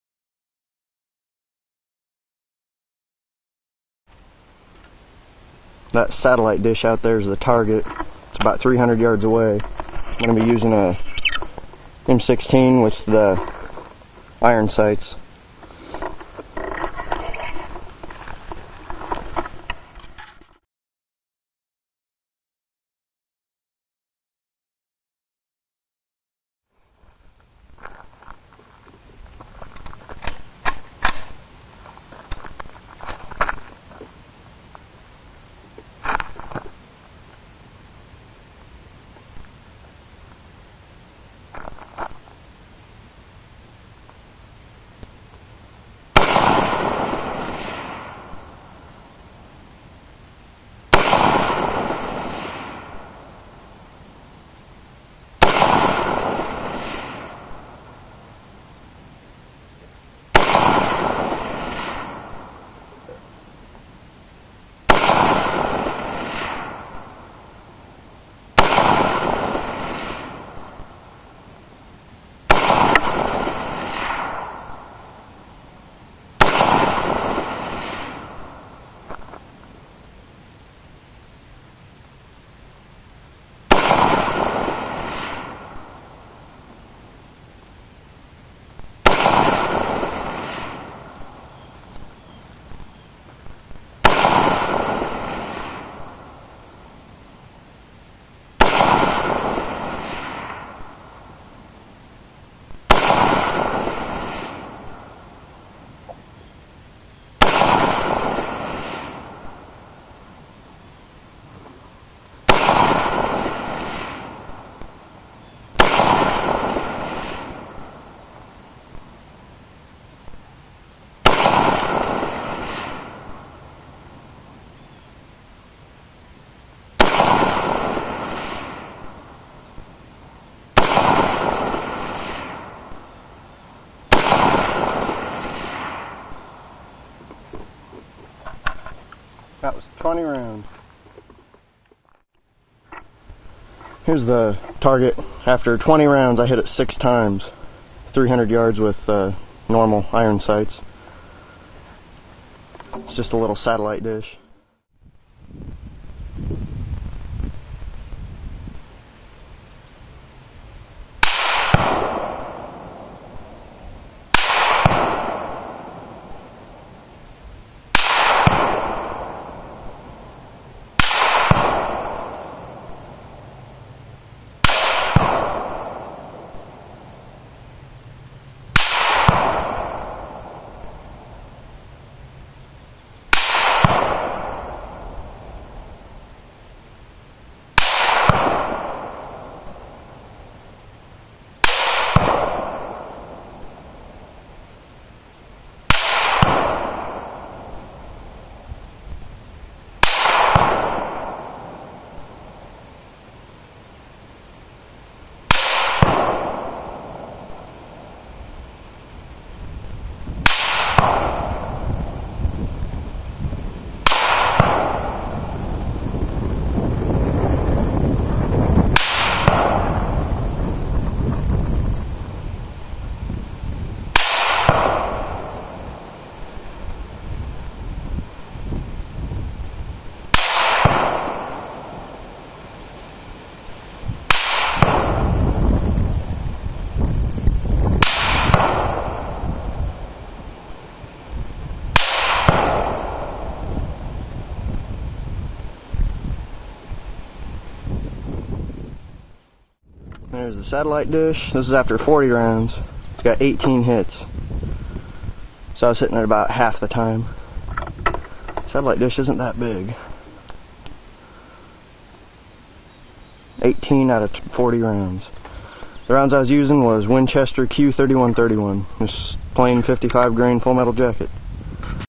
M-16 300 Yard Target Shooting
I shoot at a small satellite dish 300 yards away with my M-16 A3! The actual range was about 320 yards, but I rounded down. I got 18 out of 40 hits making a 45% hit probability on this small, distant target with Winchester Q-3131 55-grain FMJ-BT (Full Metal Jacket-Boat Tail) factory loaded ammunition!